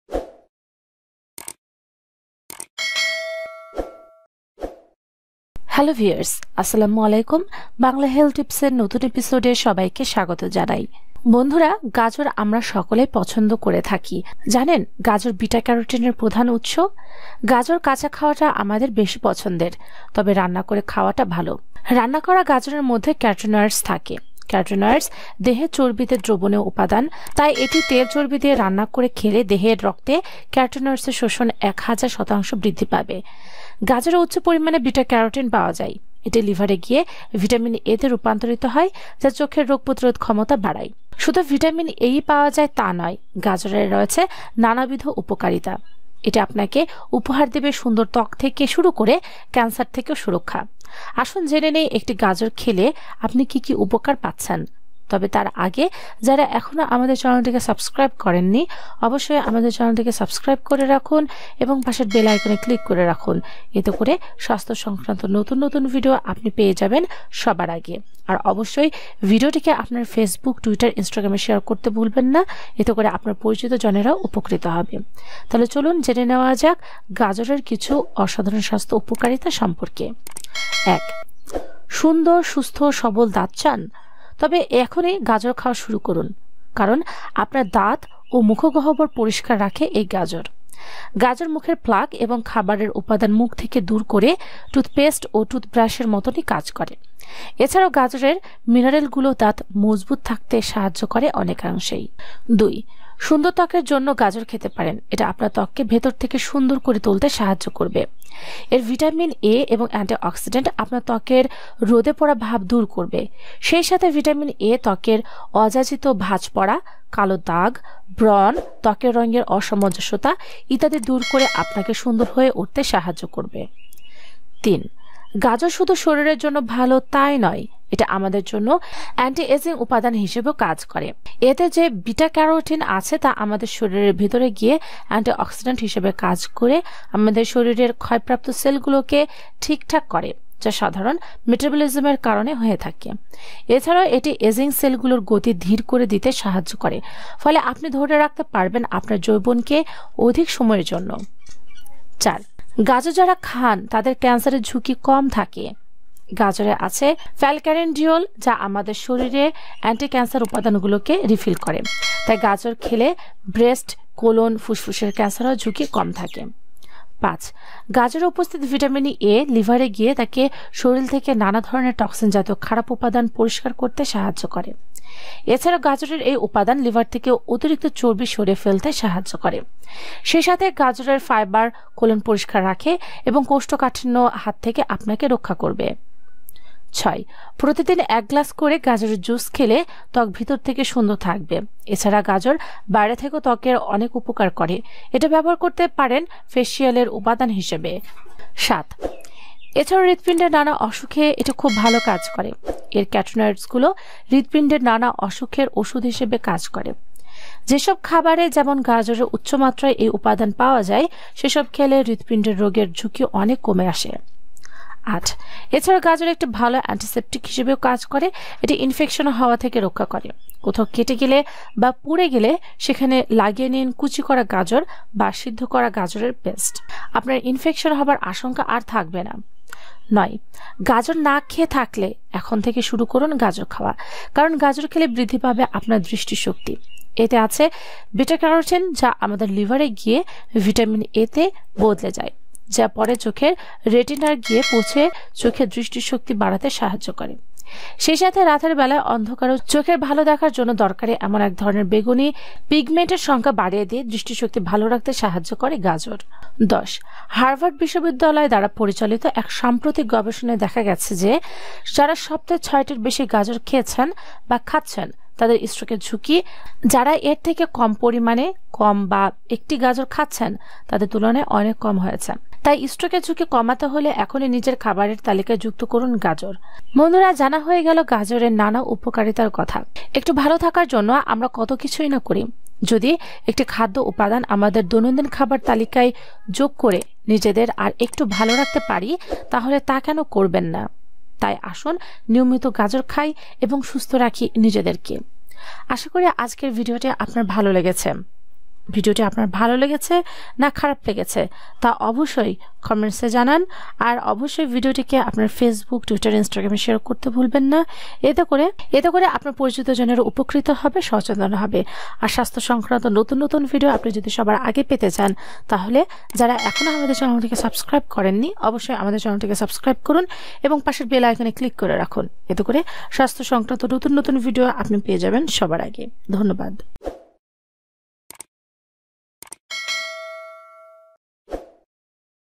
জেনে নিন গাজরের অসংখ্য না জানা গুণাবলী | গাজরের উপকারিতা ও গাজর এর ঔষধি গুনাগুন bangla health tips
১. google.com সুন্দর ও সুস্থ্য সবল দাঁত চান? তবে এখনি গাজর খাওয়া শুরু করুন। গাজর আপনার দাঁত ও মুখ গহ্বর পরিষ্কার রাখে। গাজর মুখের প্ল্যাক ও খাবারের উপাদান মুখ থেকে দূর করে টুথ পেস্ট ও টুথ ব্রাশের মতই। এছাড়াও গাজরের মিনারেলগুলো দাঁত মজবুত থাকতে সাহায্য করে অনেকাংশেই।

২. youtube.com সুন্দর ত্বকের জন্যও গাজর খেতে পারেন। এটা আপনার ত্বককে ভেতর থেকে সুন্দর করে তুলতে সাহায্য করবে। এর ভিটামিন এ ও এন্টিওক্সিডেন্ট আপনার ত্বকের রোদে পোড়া ভাব দুর করবে। সেই সাথে ভিটামিন এ ত্বকের অযাচিত ভাঁজ পড়া, কালো দাগ, ব্রন, ত্বকের রঙের অসামাঞ্জসসতা ইত্যাদি দূর করে আপনাকে সুন্দর হয়ে উঠতে সাহায্য করবে।